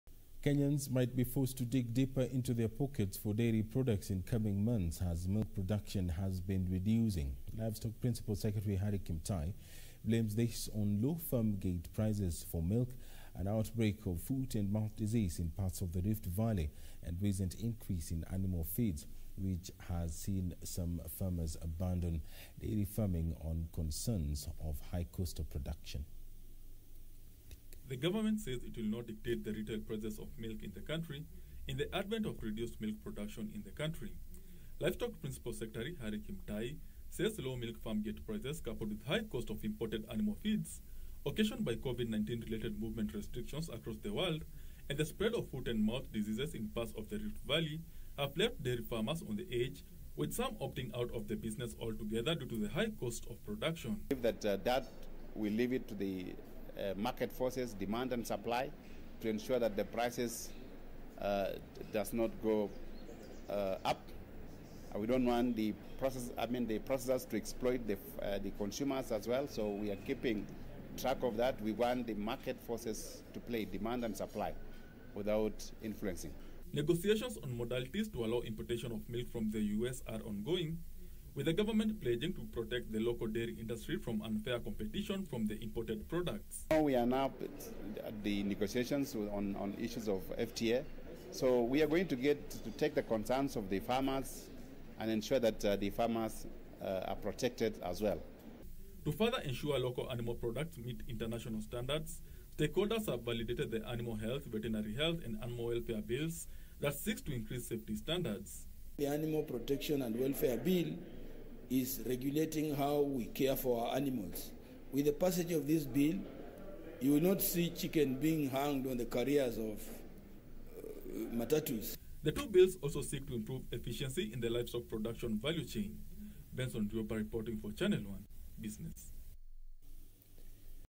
Cost of dairy products expected to increase
Kenyans might be forced to dig deeper into their pockets for dairy products in coming months as milk production has been reducing. Livestock Principal Secretary Harry Kimtai blames this on low farm gate prices for milk, an outbreak of foot and mouth disease in parts of the Rift Valley and recent increase in animal feeds, which has seen some farmers abandoning dairy farming on concerns of high cost of production.